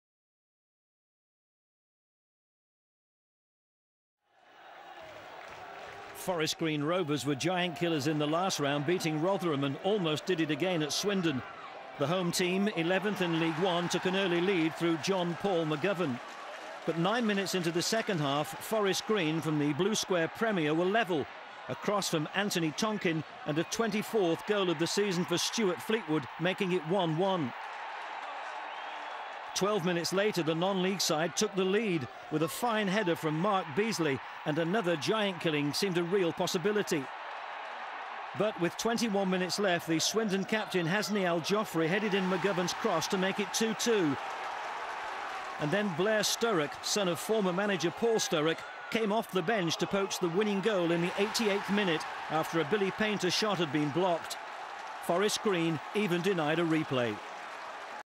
2007-12-01 Swindon Town vs Forest Green Rovers
Swindon survive a scare against Conference outfit Forest Green Rovers - after going behind midway through the second half, goals from Aljofree & Sturrock spare the Town's blushes.